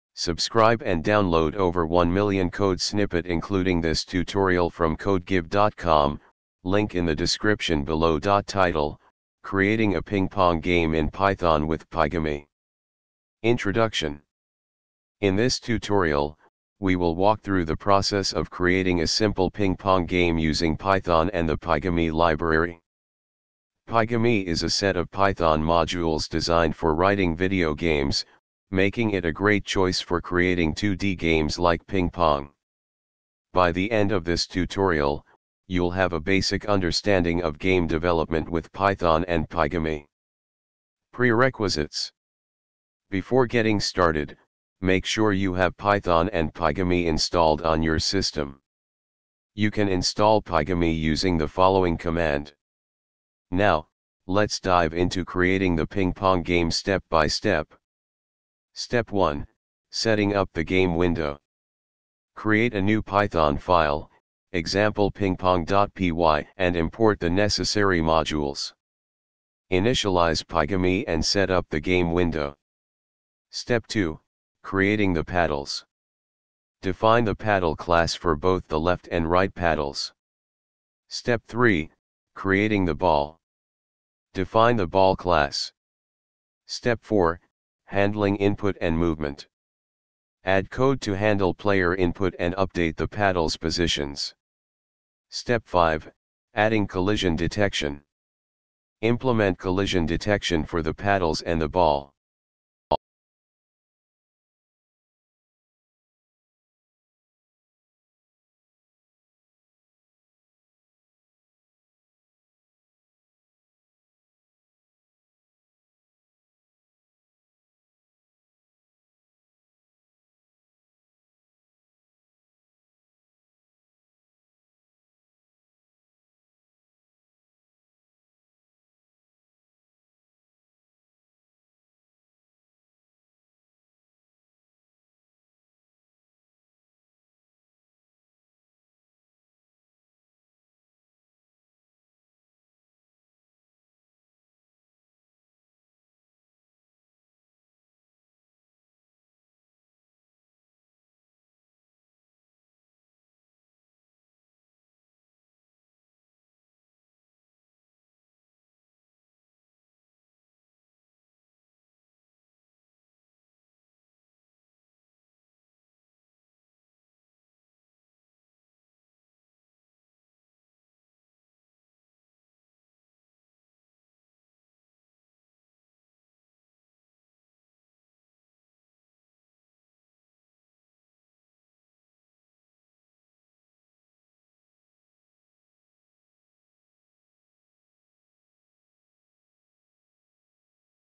python code for ping pong game
title: creating a ping pong game in python with pygame
introduction:
in this tutorial, we will walk through the process of creating a simple ping pong game using python and the pygame library. pygame is a set of python modules designed for writing video games, making it a great choice for creating 2d games like ping pong. by the end of this tutorial, you'll have a basic understanding of game development with python and pygame.
prerequisites:
before getting started, make sure you have python and pygame installed on your system. you can install pygame using the following command:
now, let's dive into creating the ping pong game step by step.
step 1: setting up the game window
create a new python file (e.g., pingpong.py) and import the necessary modules.
initialize pygame and set up the game window.
step 2: creating the paddles
define the paddle class for both the left and right paddles.
step 3: creating the ball
define the ball class.
step 4: handling input and movement
add code to handle player input and update the paddles' positions.
step 5: adding collision detection
implement collision detection for the paddles and the ball.